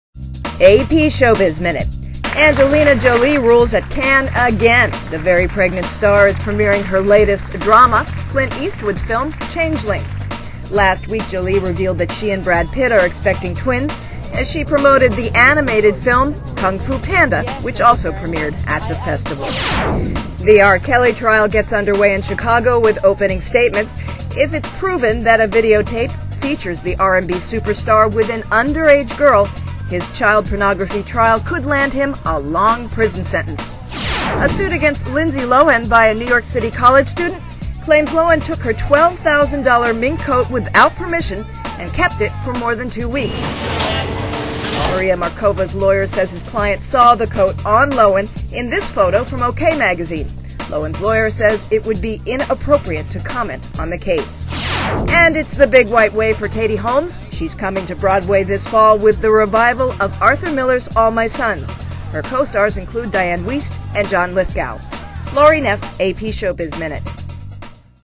ShowBiz Minute: Jolie, R. Kelly, Lohan, Holmes
EntertainmentEntertainment ExtraShowBiz Minute: Jolie, R. Kelly, Lohan, HolmesShowBiz Minute: Jolie, R. Kelly, Lohan, HolmesThe Associated PressAngelina Jolie, Clint Eastwood premiere 'Changeling' at Cannes; Opening statements at R. Kelly trial; Lohan accused of taking mink coat; Katie Holmes is coming to Broadway. (May 20)[Notes:ANCHOR VOICE] AP ShowBiz Minute.Angelina Jolie rules at Cannes - again. The very pregnant star is premiering her latest drama - Clint Eastwood's film, 'Changeling.' It's the true story of an LA woman and her crusade to find out what happened to her son after he was kidnapped in 1928.Last week Jolie revealed that she and Brad Pitt are expecting twins - as she promoted the animated film, 'Kung Fu Panda,' which also premiered at the festival.The R. Kelly trial gets under way in Chicago with opening statements. If it's proven that a videotape features the R&B superstar and an underage girl, his child pornography trial could land him a lengthy prison sentence.A suit against Lindsay Lohan by a New York City college student claims Lohan took her 12,000-dollar mink coat without permission - and kept it for more than 2 weeks.Maria Markova's lawyer says his client saw the coat on Lohan in this photo from OK! magazine. Lohan's lawyer says it would be inappropriate to comment on the case.And it's the Big White Way for Katie Holmes. She's coming to Broadway this fall with the revival of Arthur Miller's 'All My Sons.' Her co-stars include Dianne Wiest and John Lithgow. ___ ___, The Associated Press.( END ) ANCHOR VOICE: Lauri Neff VIDEO PRODUCER: Neff VIDEO SOURCE: AP, OK! magazine VIDEO APPROVAL: N.M. VIDEO RESTRICTIONS:Courtesy